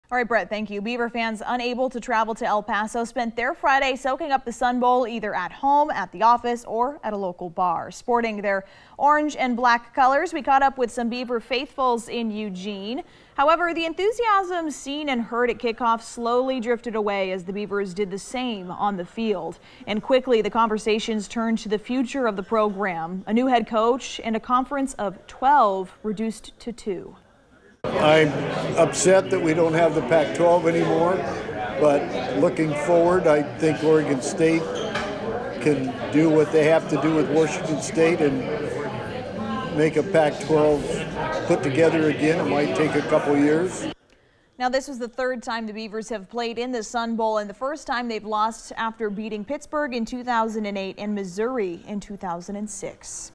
Beavers fans soak up the Sun Bowl in Eugene
Oregon State fans gathered at a local bar in Eugene to watch the Beavs’ in the Sun Bowl. As the enthusiasm faded with OSU losing, Beaver faithful looked ahead to a future of change.